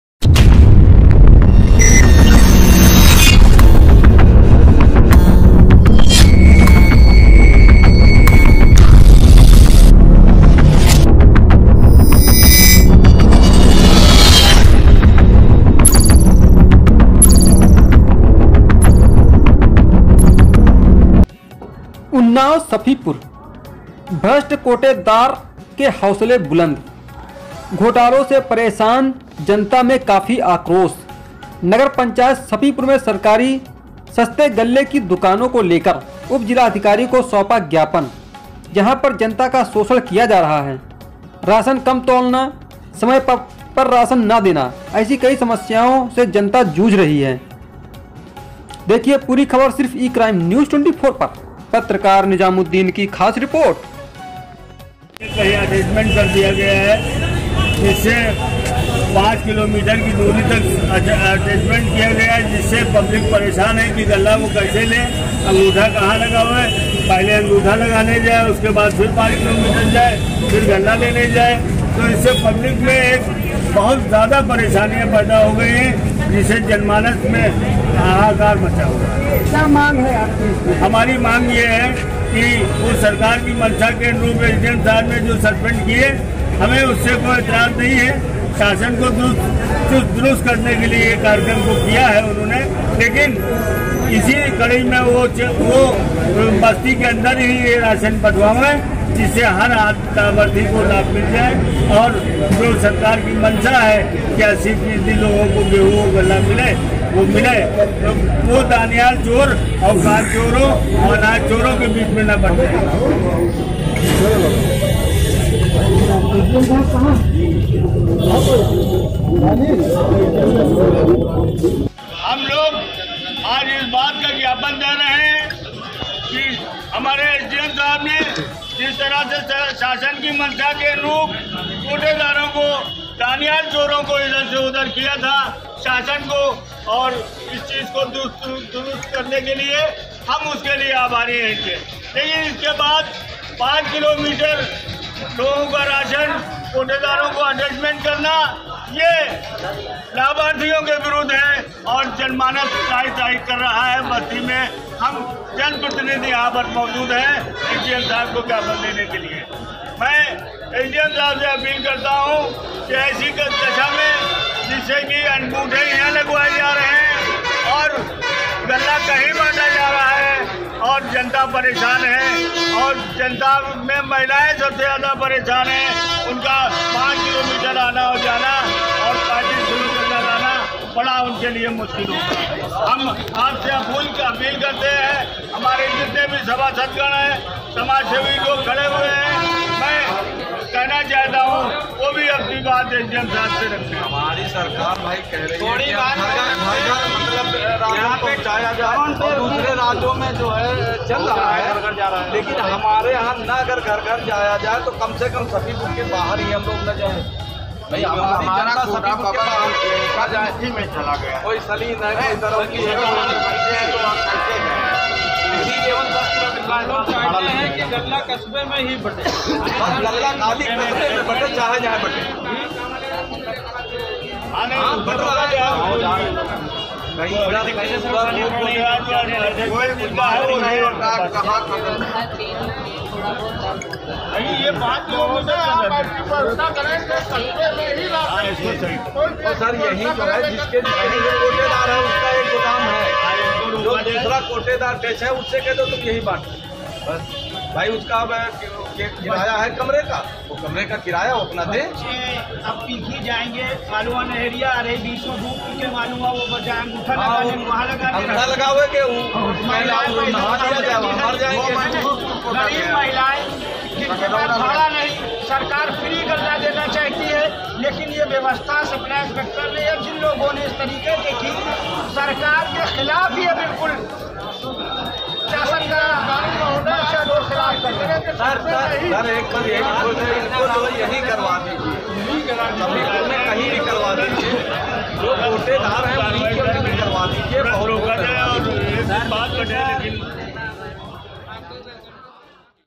उन्नाव,Safipur भ्रष्ट कोटेदारों के हौसले बुलंद घोटालो से परेशान जनता में काफी आक्रोश SDM ने दिखाएं
Ecrime News 24,UP Live:उन्नाव,सफीपुर भ्रष्ट कोटेदारों के हौसले बुलंद घोटालो से परेशान जनता में काफी आक्रोश SDM ने दिखाएं तेवर